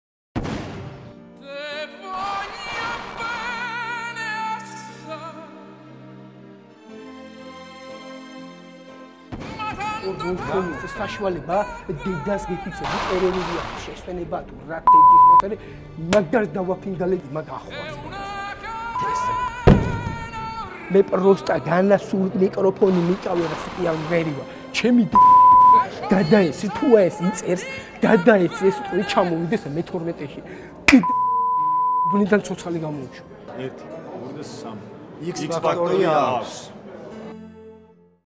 გაბრაზებული კონკურსანტი
ჟიურის შეფასებით გაბრაზებული კონკურსანტი.

იხილეთ სრულად ორშაბათს, 14 აპრილს, 22 საათზე, რუსთავი 2-ს ეთერში!